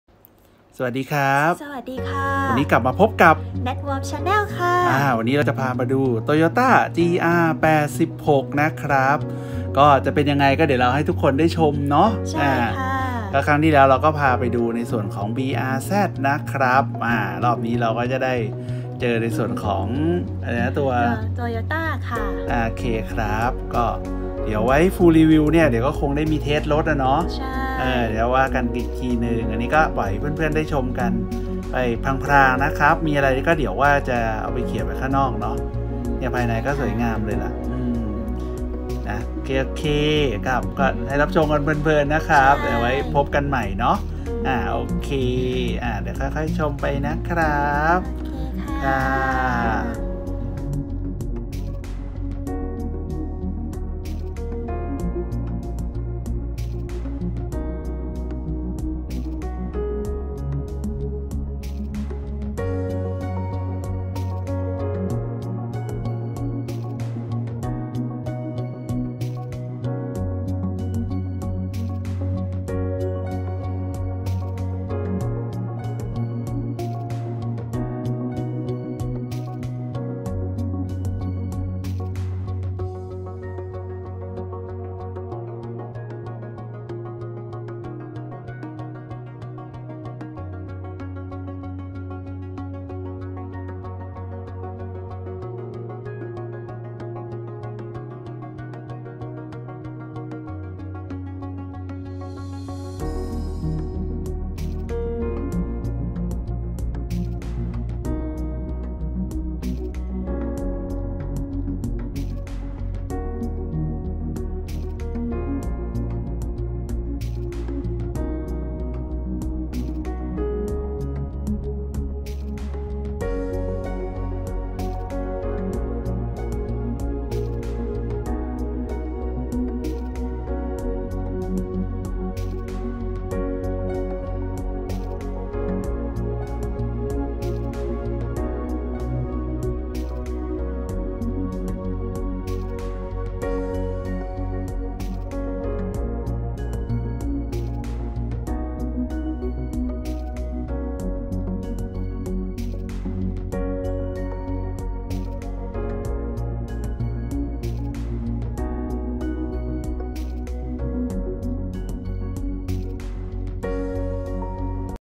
Cool!!! TOYOTA GR86
โฉบเฉี่ยว โดดเด่น ไปกับ TOYOTA GR86

ขอขอบคุณทาง GLAM ที่ให้โอกาสพวกเราได้มารีวิวเบื้องต้นกันนะครับ//ค่ะ